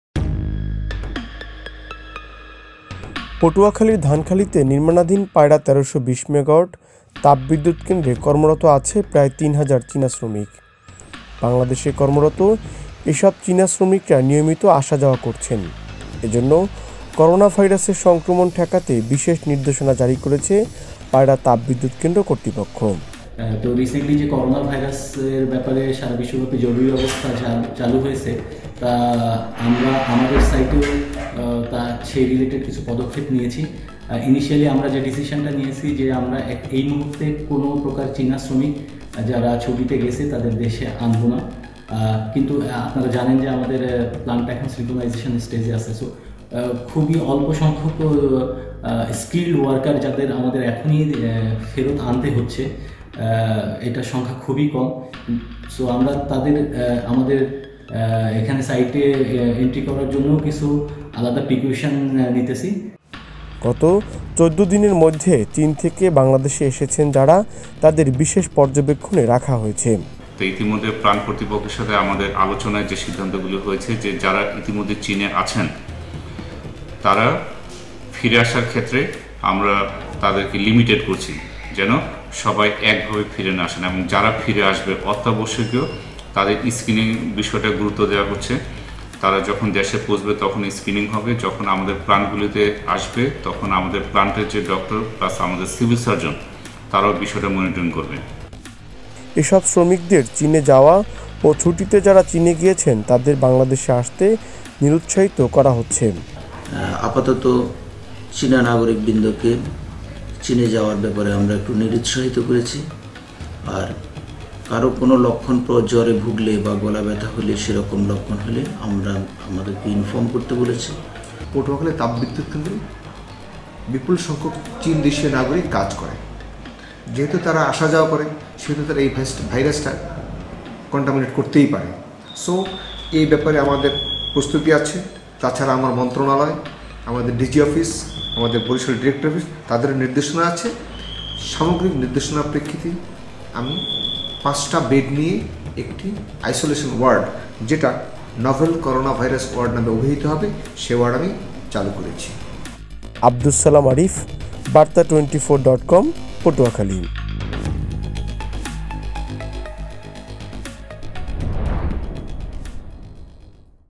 করোনাভাইরাস ঠেকাতে পায়রা তাপ বিদ্যুৎ কেন্দ্রে সতর্কতা | Barta24.com
পটুয়াখালীর ধানখালীতে নির্মানাধীন পায়রা ১৩২০ মেগাওয়াট  তাপ বিদ্যুৎ কেন্দ্রে কর্মরত আছে প্রায় ৩ হাজার চীনা শ্রমিক। বাংলাদেশে কর্মরত এসব চীনা শ্রমিকরা নিয়মিত আসা-যাওয়া করছেন। এজন্য করোনাভাইরাসের সংক্রমণ ঠেকাতে বিশেষ নির্দেশনা জারি করেছে পায়রা তাপ বিদ্যুৎ কেন্দ্র কর্তৃপক্ষ।